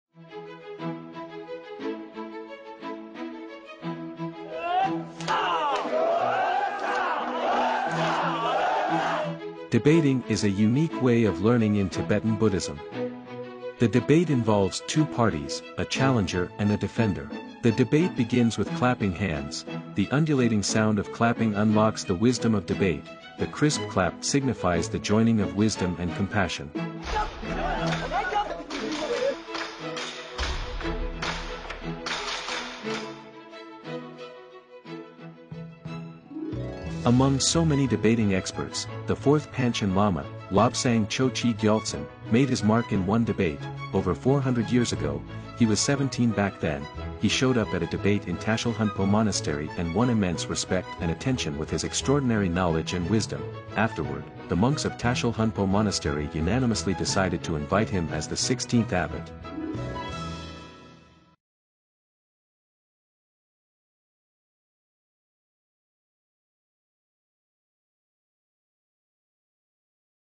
They're not fighting! Check how the monks of Tashilhunpo Monastery debate
Debating is a unique way of learning in Tibetan Buddhism. The debate involves two parties, a challenger and a defender. The debate begins with clapping hands. The undulating sound of clapping unlocks the wisdom of debate.